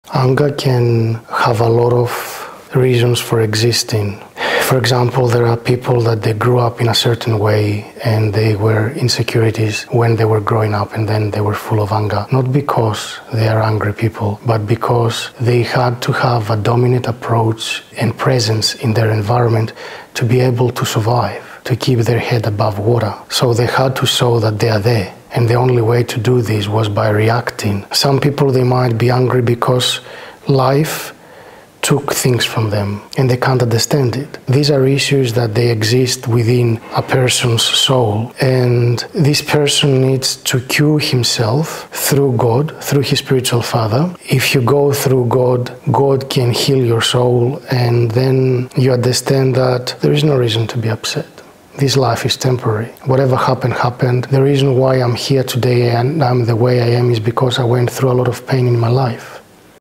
Understanding Anger | Bishop Emilianos
Anger can have a lot of reasons for existing. In this clip, Bishop Emilianos helps us to look beyond the behaviour and to the source, to better understand ourselves and our fellow man.